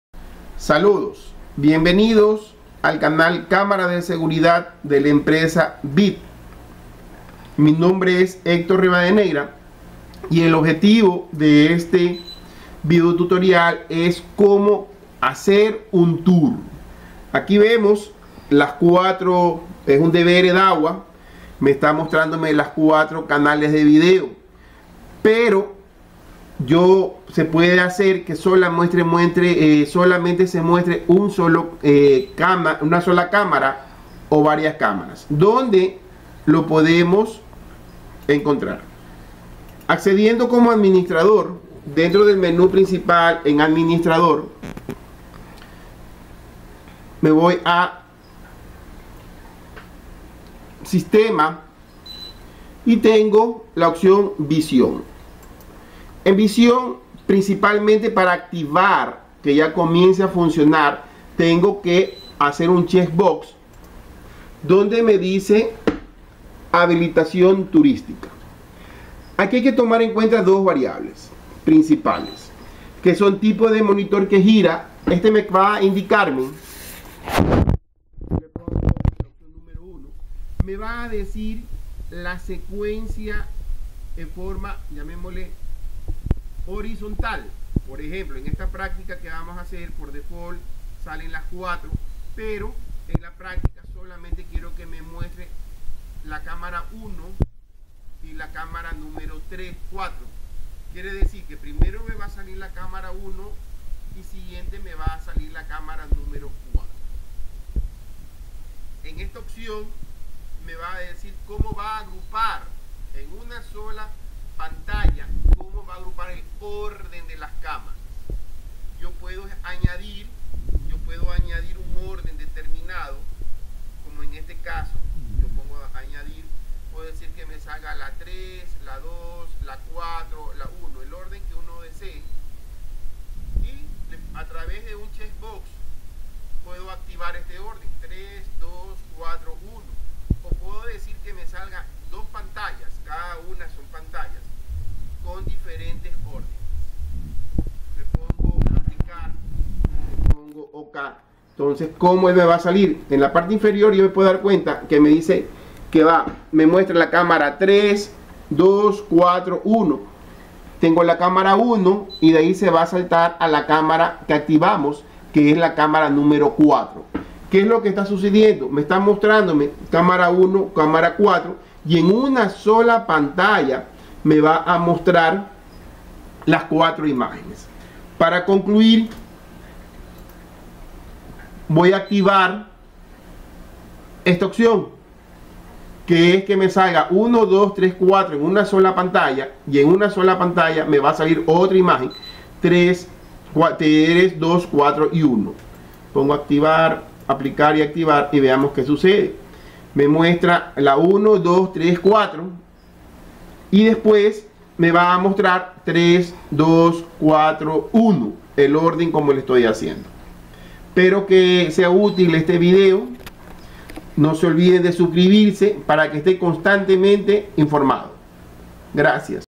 04 Firmware 3.2 Dahua - Tour
Como configurar un Tour en un dvr dahua?

Considerando el Tipo de monitor de gira y split

Camaras de seguridad, vendemos, instalamos y configuramos en su celular, table etc.

Nos puedes contactar: (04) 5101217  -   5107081

Guayaquil - Ecuador